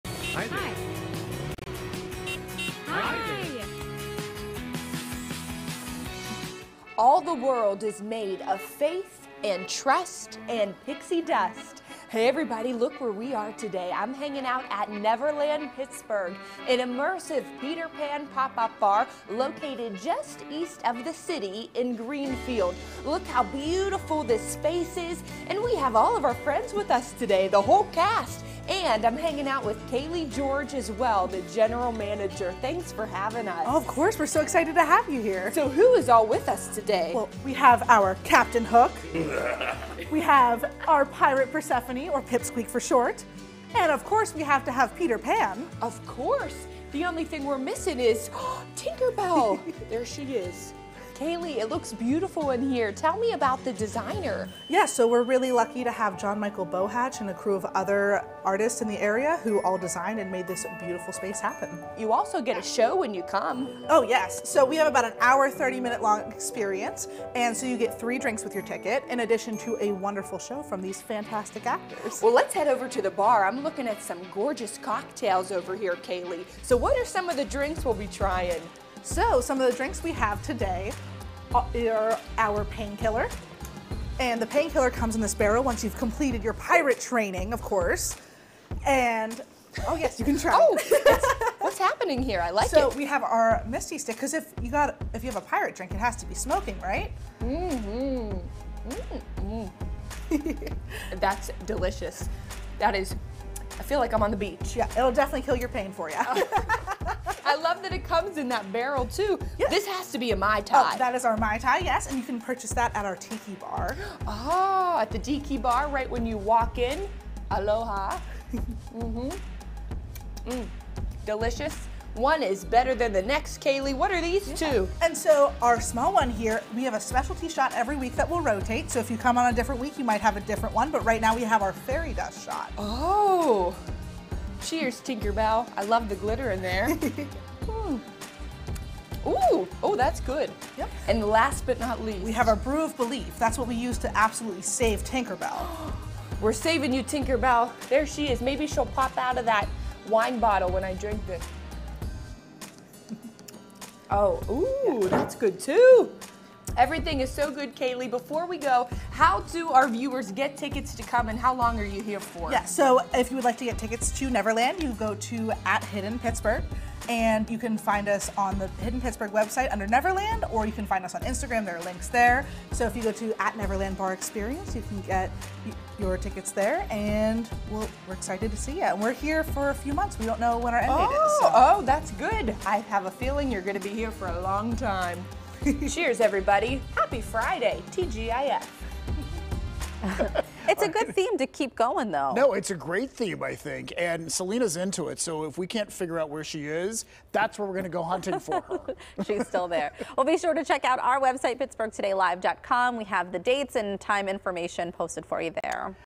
Celina Around Town: Neverland Pop-Up Bar Opens In Pittsburgh
PTL's Celina Pompeani visits Never, Neverland, or, well, the Neverland Pop-Up Bar in Greenfield to meet Peter Pan, Captain Hook and the gang!